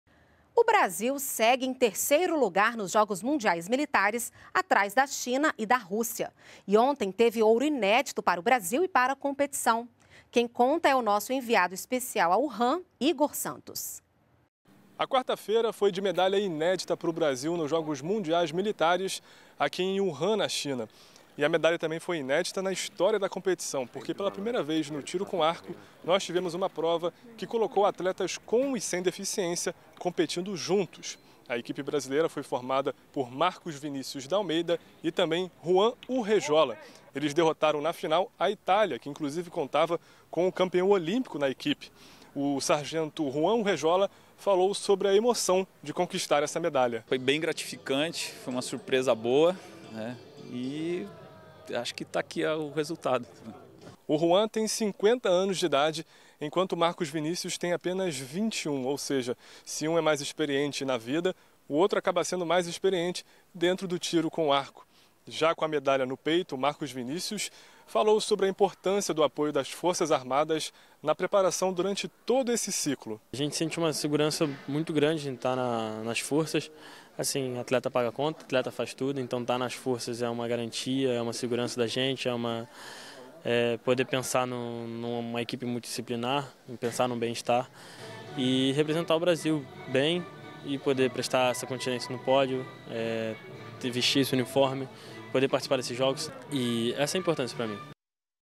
Brasil está entre os primeiros nos Jogos Mundiais Militares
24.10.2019 - O Brasil segue em terceiro lugar nos Jogos Mundiais Militares, atrás da China e da Rússia. E ontem  teve OURO inédito para o Brasil e para a competição. Quem conta é o nosso enviado especial a Urãn, Igor Santos!